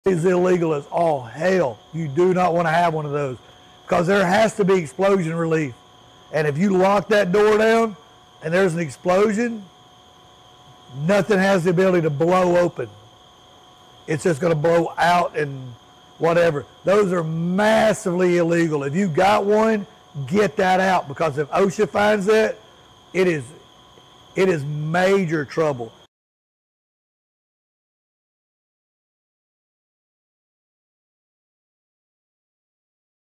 Understanding the Dangers of Certain Oven Door Latches | Joey Golliver
Join Joey Golliver, the world-renowned powder coating expert and author of "The Powder Coach's Playbook," as he explains the potential dangers associated with certain styles of oven door latches in this critical video.

In this tutorial, Joey delves into the importance of oven door safety and highlights specific latch designs that can pose risks to operators and equipment. Learn about common issues such as accidental openings, inadequate sealing, and heat loss due to faulty or outdated latch mechanisms. Joey also discusses best practices for oven door latch selection, maintenance, and safety protocols to minimize risks and ensure a secure oven environment.

Whether you're a powder coating professional or a facility manager, understanding the dangers of improper oven door latches is essential for maintaining a safe and efficient workplace. Joey’s expert insights and practical advice will help you identify potential hazards and implement measures to enhance oven safety.

Don't compromise on safety! Hit play now to gain expert knowledge from Joey Golliver on the dangers of certain oven door latches. Let’s prioritize safety in our operations together!

Click the link below to sign up for the next Powder-X Coating Systems training.